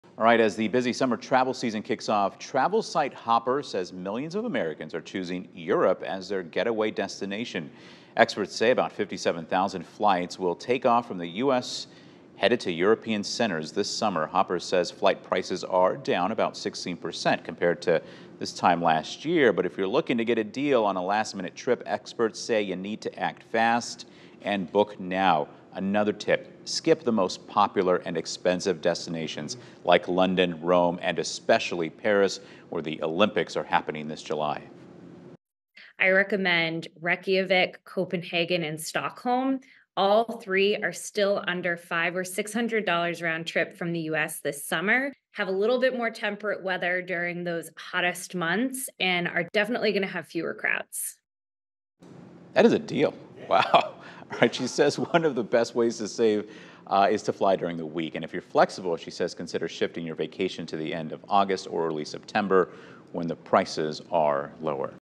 Summer travel tips for Europe
According to travel website Hopper, flight prices are down about 16% compared to 2023.